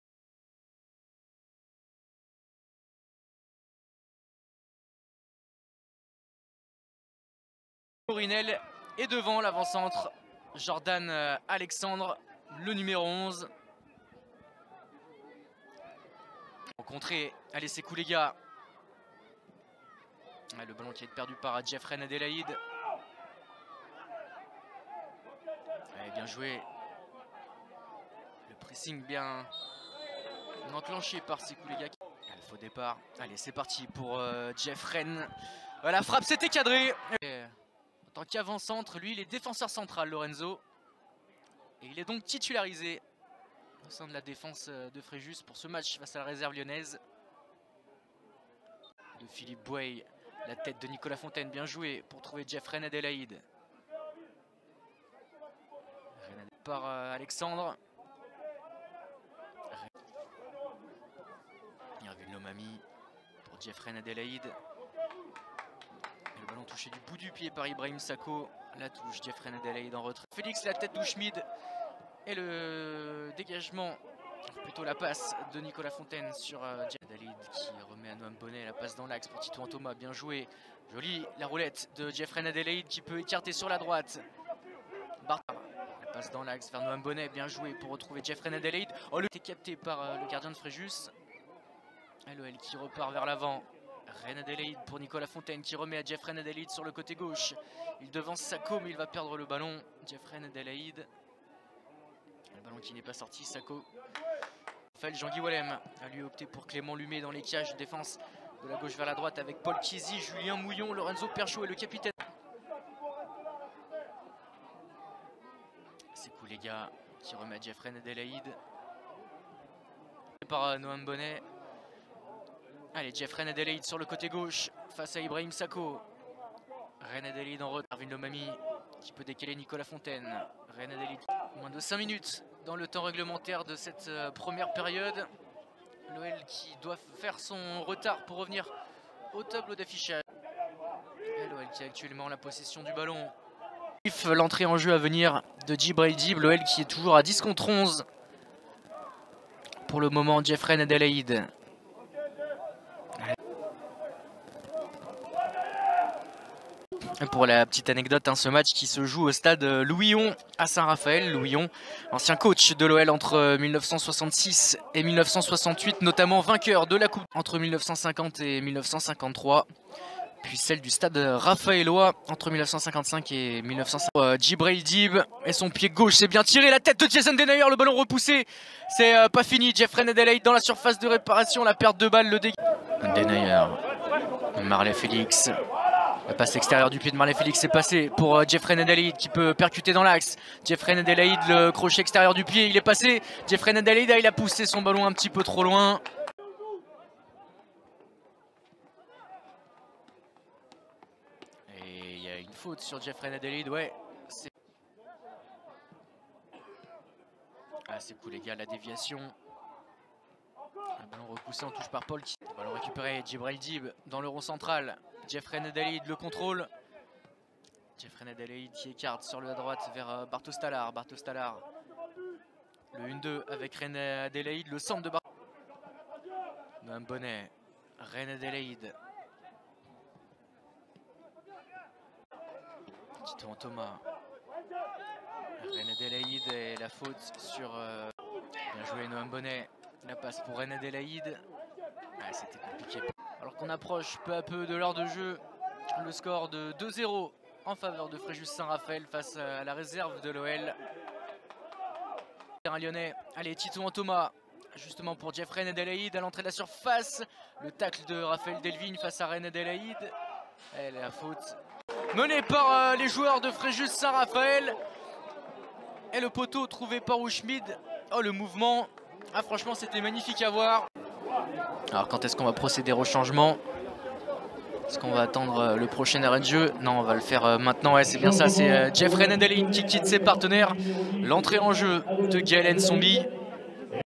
Jeff Reine Adélaïde vs Etoile Fréjus Saint-Raphaël (Retour de blessure #2)
Le deuxième match d'affilées de Jeff Reine Adelaide avec la réserve de l'OL depuis son retour de blessure. Le milieu de 24 ans a cette fois disputé 65min de la rencontre opposant l'Etoile Fréjus Saint-Raphaël à l'Olympique lyonnais.

IMAGES OLPLAY